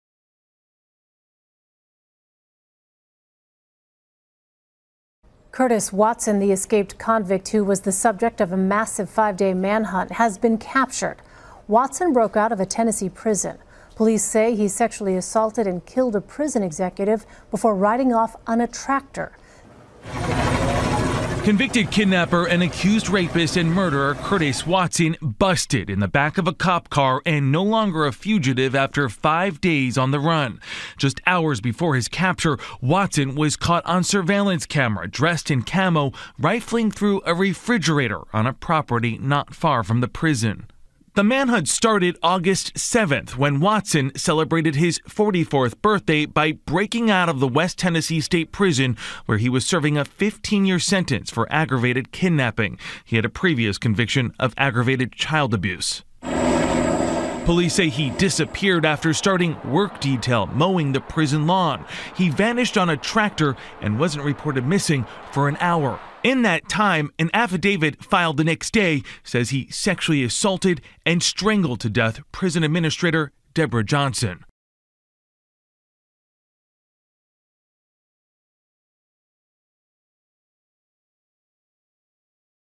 Breaking | Escaped Tennessee prisoner captured after 5-day manhunt - 11 August 2019
Perpetual Pixel's Top International News Headlines Today features breaking news, news headlines and current events happening all over the world.

► Source:
CBS News